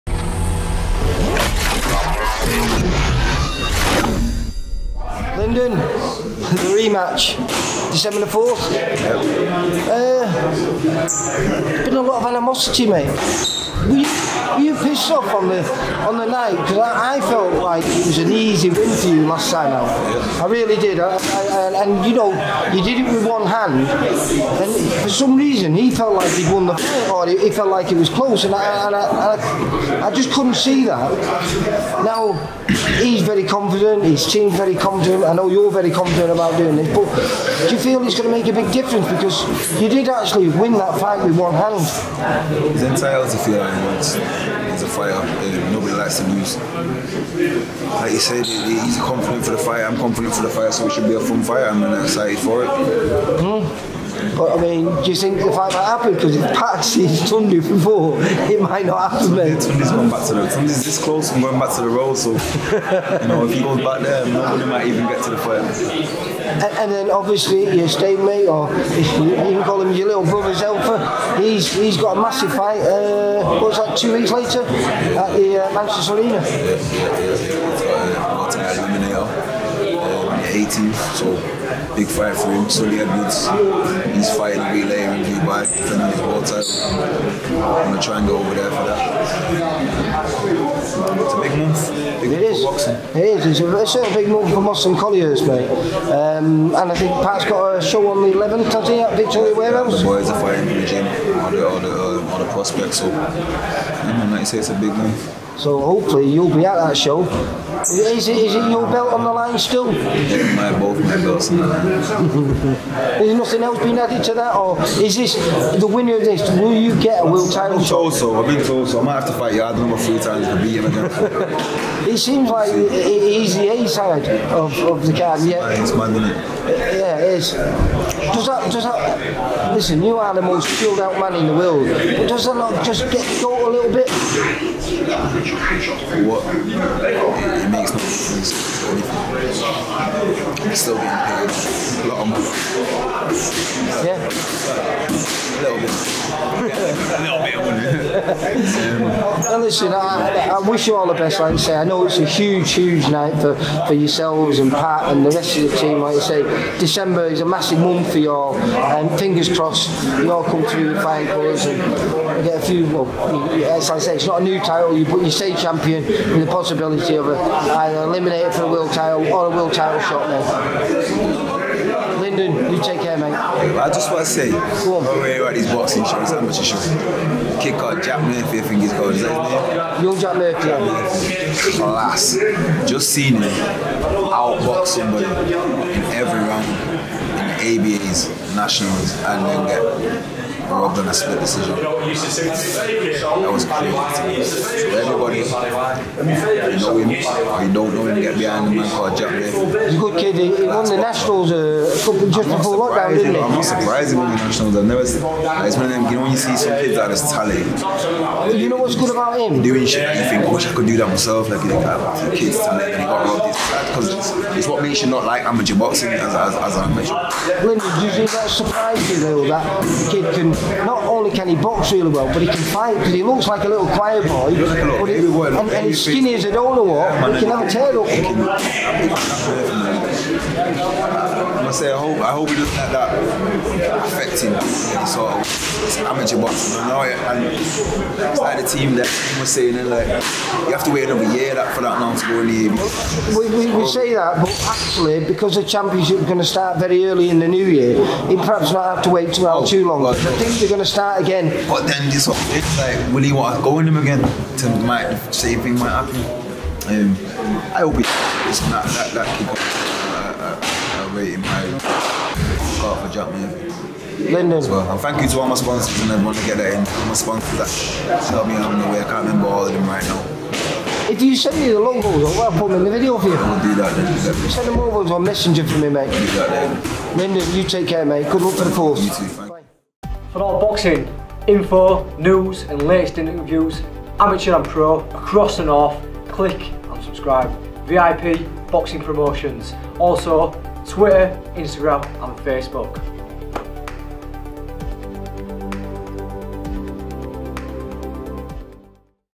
Lyndon Arthur talks rematch with Yarde, Zelfa Barrett and at busy December for Pat Barrett'
VIPtv caught up with Lyndon Arthur at the Northwest regional ABA finals where he talks rematch with Yarde, Zelfa Barrett big fight on Dec 18th and at busy December for his trainer Pat Barrett'